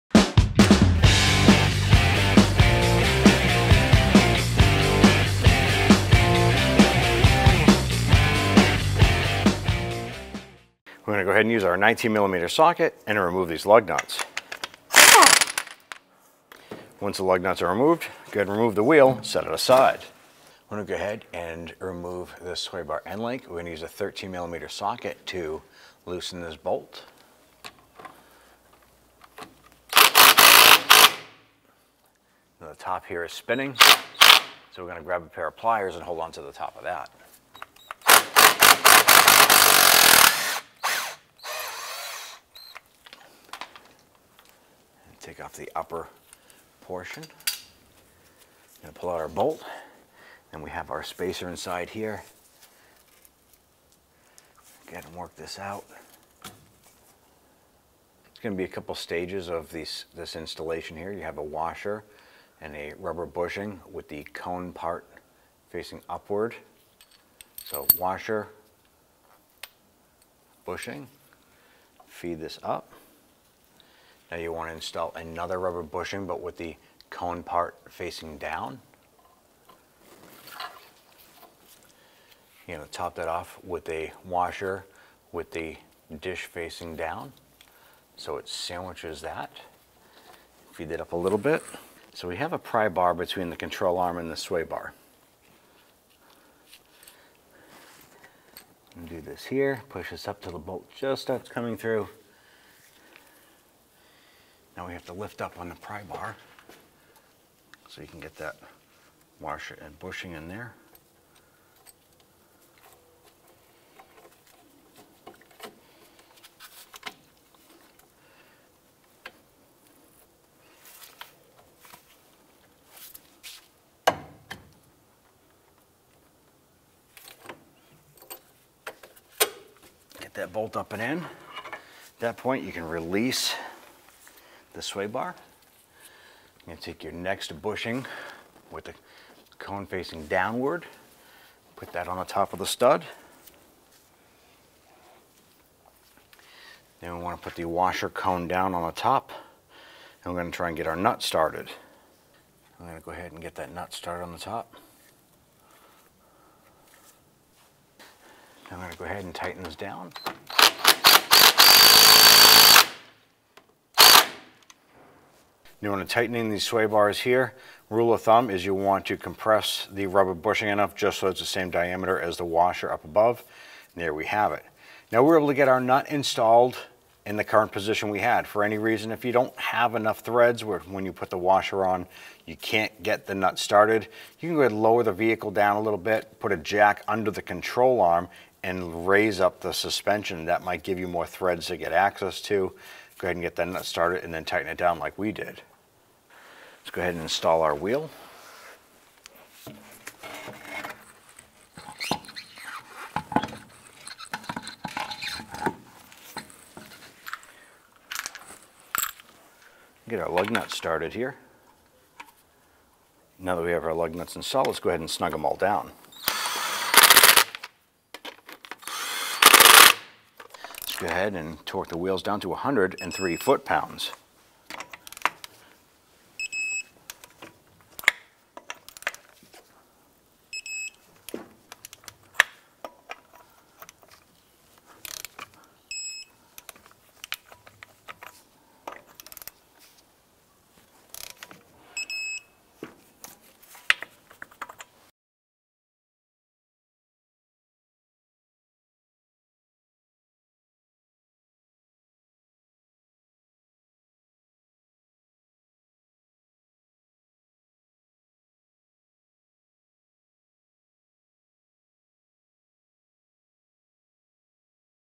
How To Replace Front Sway Bar Links 2006-2011 Cadillac DTS
Sway bar links connect the sway bar (stabilizer bar) to other suspension components. They have a movable joint that can be damaged or wear out over time. When that happens, the joints get loose, and rattle. This video shows you how to install new TRQ sway bar links on your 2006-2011 Cadillac DTS.

This repair was done on a 2010 Cadillac DTS Platinum 4.6L and the process should be similar on the following vehicles:
2006 Cadillac DTS
2007 Cadillac DTS
2008 Cadillac DTS
2009 Cadillac DTS
2010 Cadillac DTS
2011 Cadillac DTS